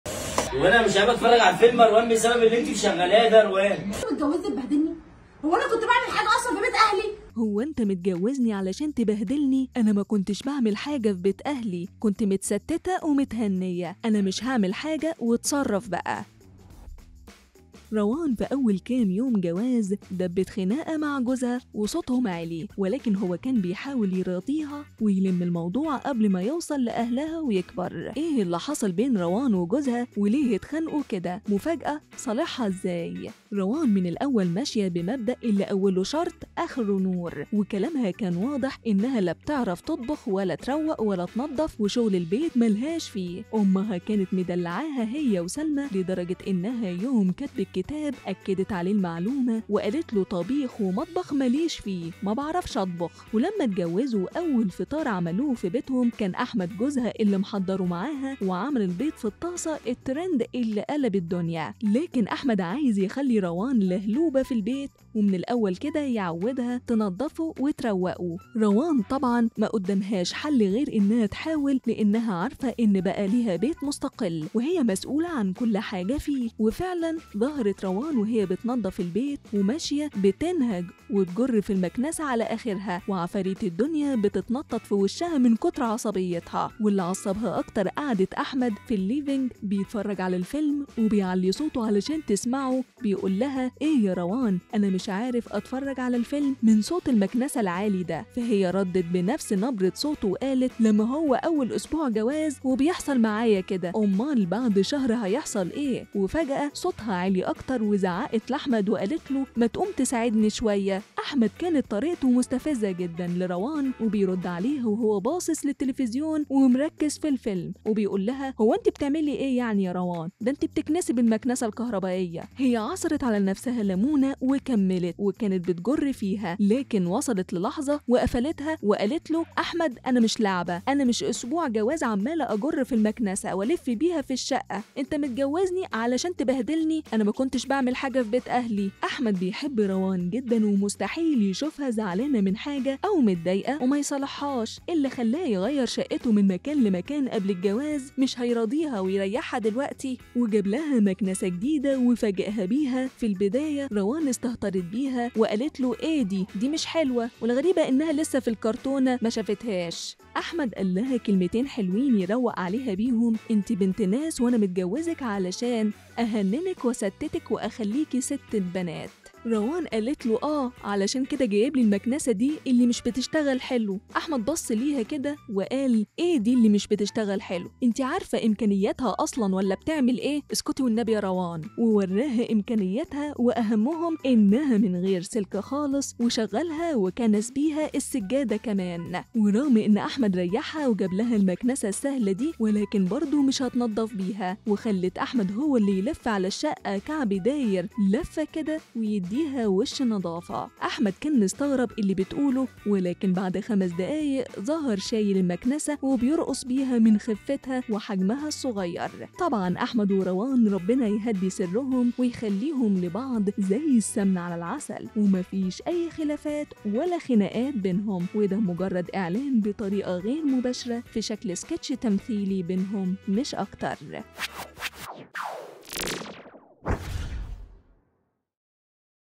أنت متجوزنى تبهدلنى؟!.. خناقة بين روان وجوزها بعد أسبوع جواز: إيه اللى حصل بينهم؟ وإزاى صالحها
روان من الأول ماشية بمبدأ اللى أوله شرط أخره نور .. وكلامها كان واضح  إنها لا بتعرف تطبخ ولا تروق ولا تنضف وشغل البيت ملهاش فيه.. أمها كانت مدلعاها هى وسلمى لدرجة إنها يوم كتب الكتاب أكدت عليه المعلومة وقالت له طبيخ ومطبخ مليش فيه مبعرفش أطبخ! ولما اتجوزوا اول فطار عملوه فى بيتهم كان احمد جوزها اللى محضره معاها وعمل البيض فى الطاسة التريند اللى قلبت الدنيا!